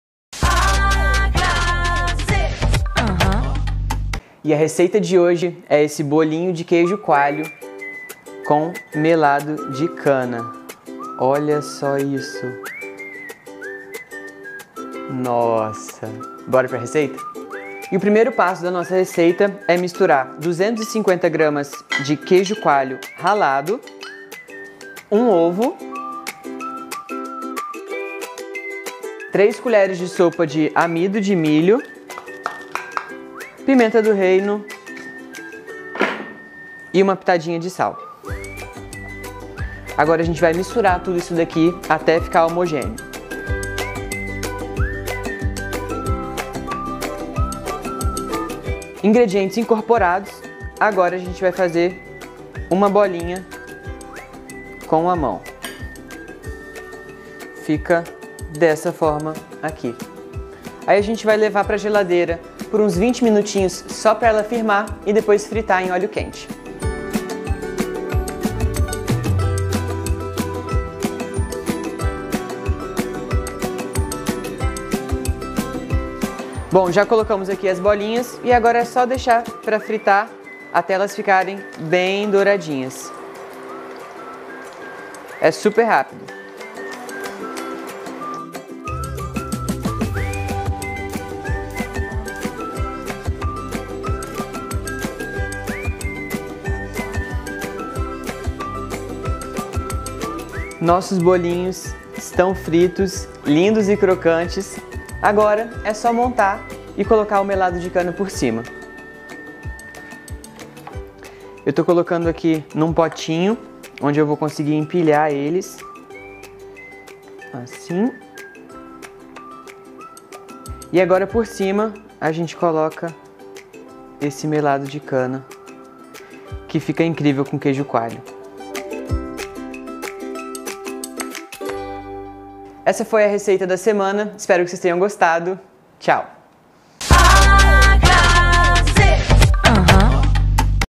Bolinha de queijo coalho com melado de cana
O chef e sommelier Pedro Kucht ensina a preparar bolinhos de queijo coalho crocantes e irresistíveis. Para acompanhar o petisco, ele sugere melado de cana.